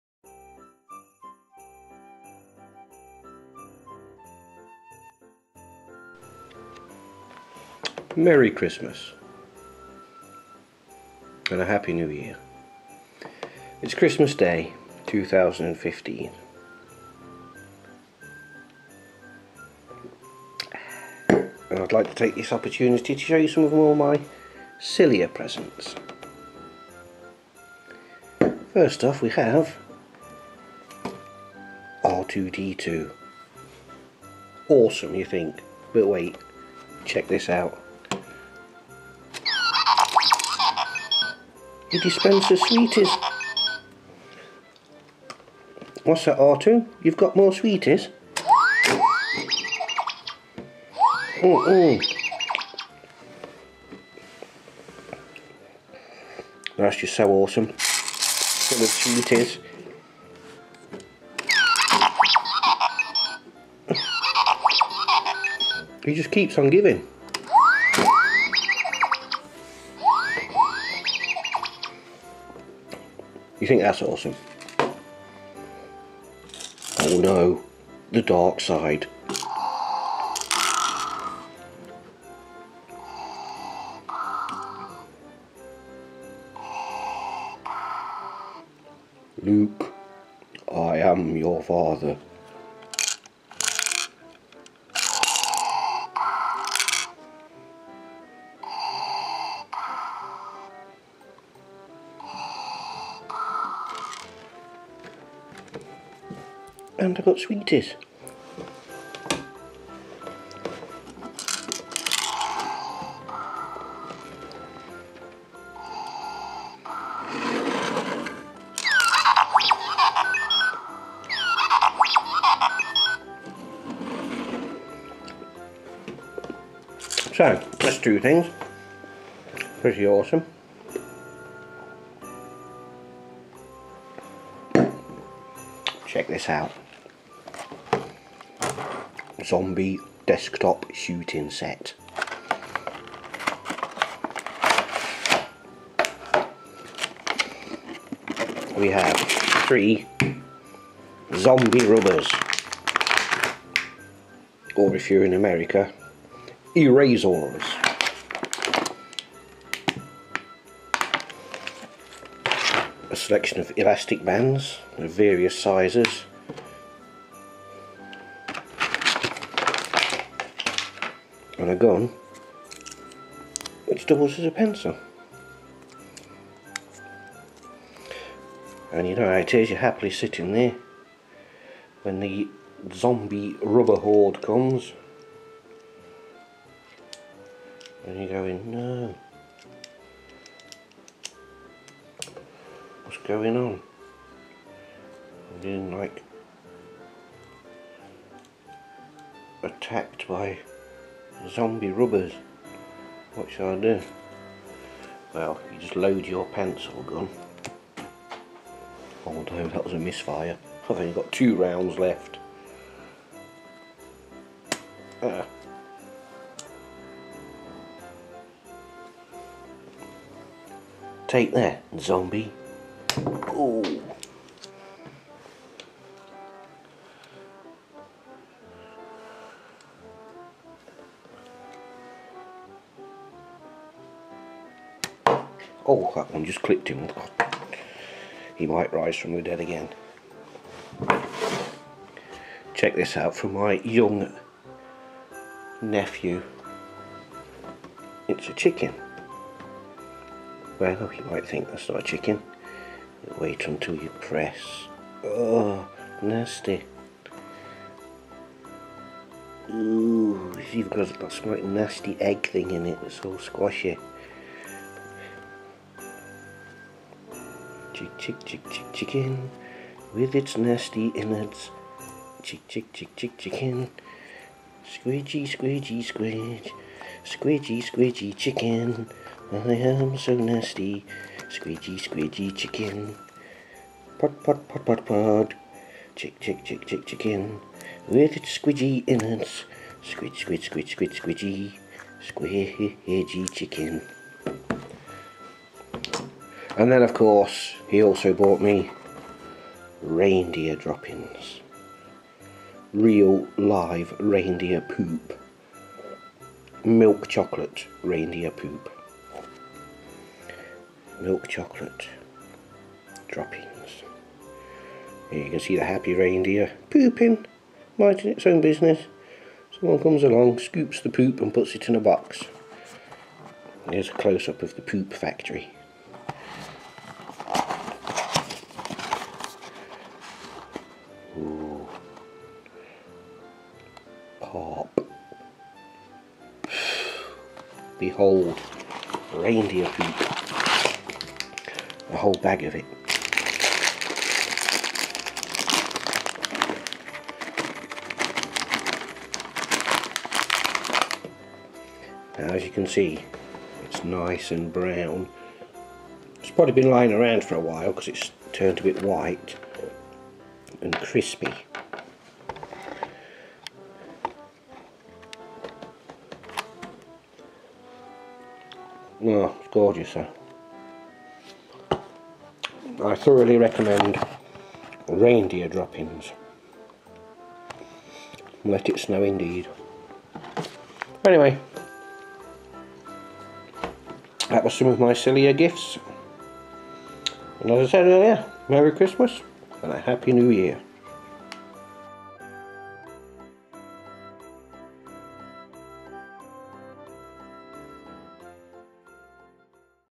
Silly Christmas Presents 2015
Some presents I got for Christmas that are a bit of fun.